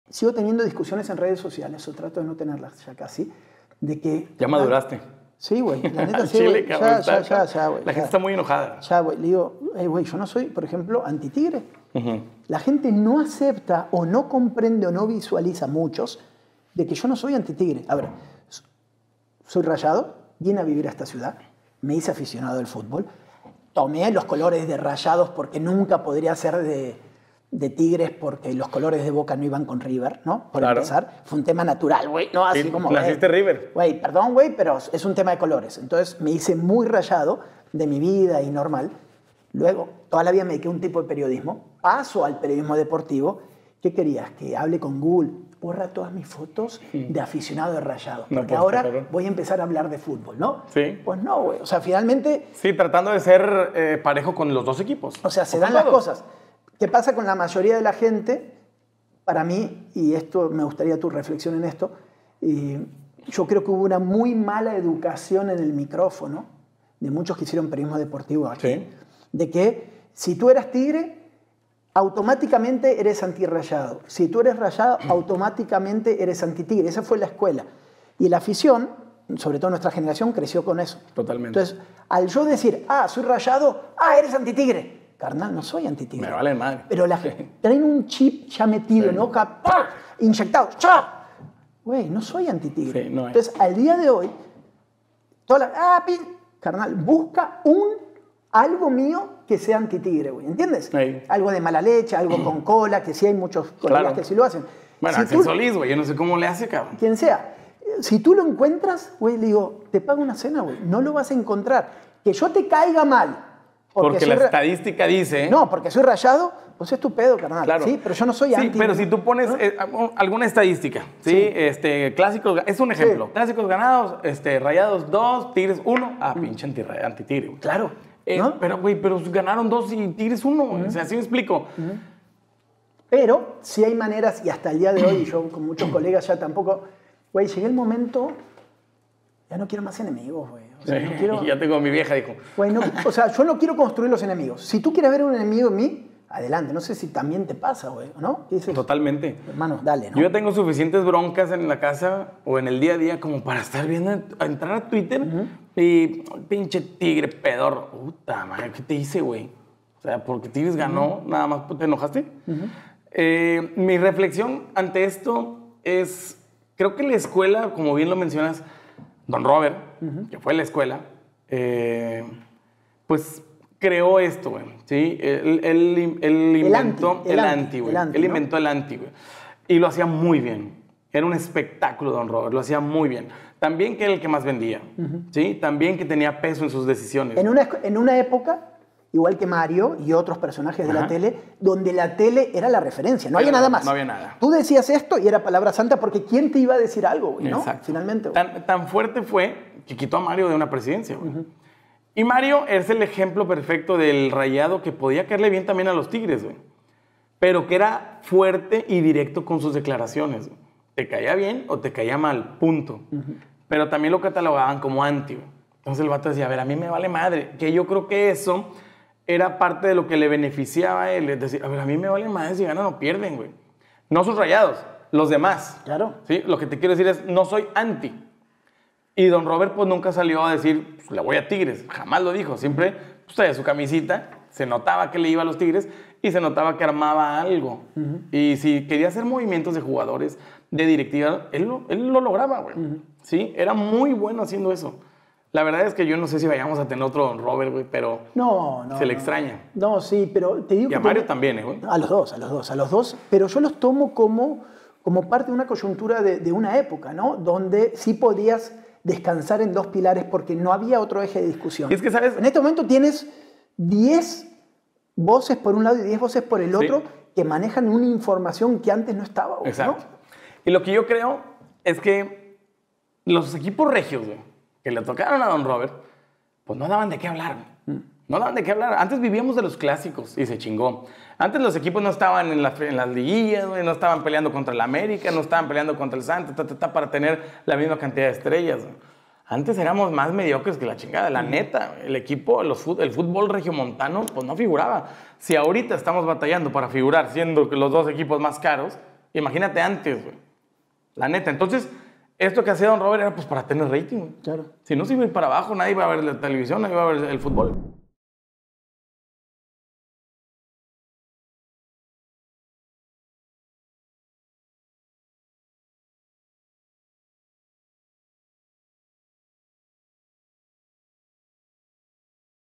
DON ROBER QUITÓ A CASTILLEJOS DE LA PRESIDENCIA - Rudy (PARADELANTISMO)
⚽️ Fútbol Prohibido 🚫

🎙️ Podcast de Entrevistas: En su serie de podcasts, Santiago conversa con figuras clave, rompiendo estigmas y explorando temas sociales profundos.